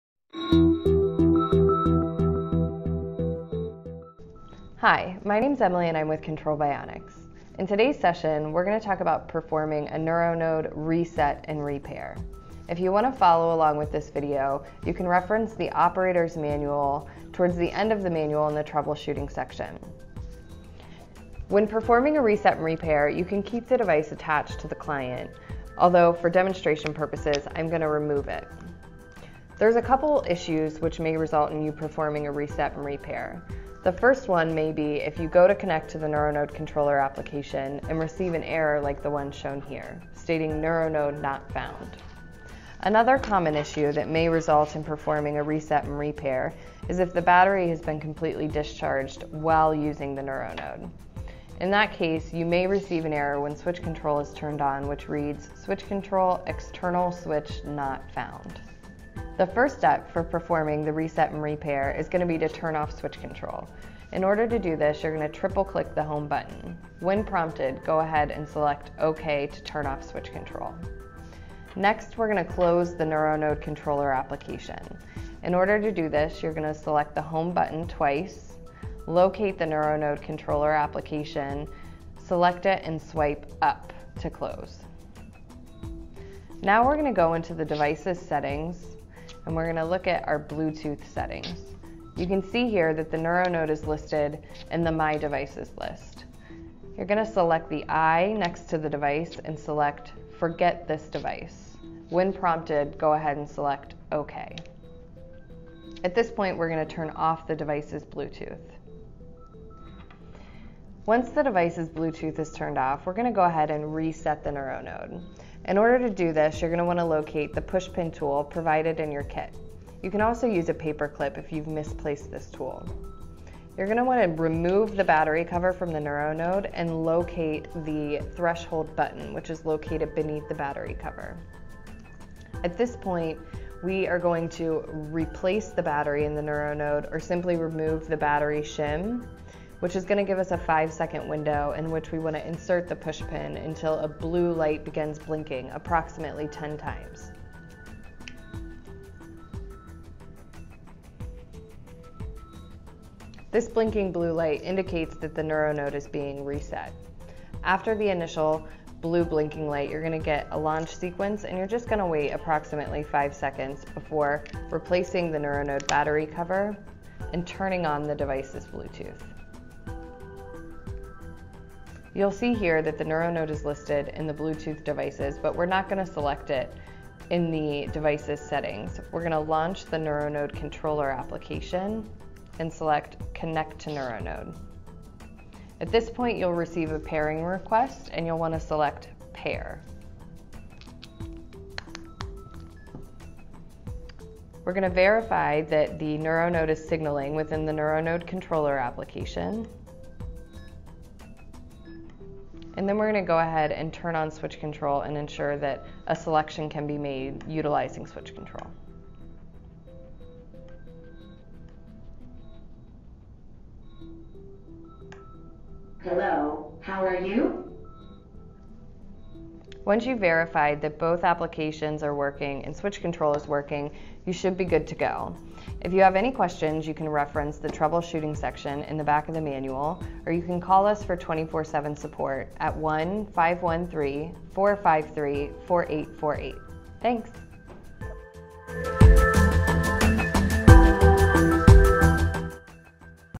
How To Reset and Re-Pair the NeuroNode
Watch this brief video to see a demonstration of the NeuroNode's reset and re-pair procedure. You can follow along, or review this process, in the Operator's Manual in the troubleshooting section.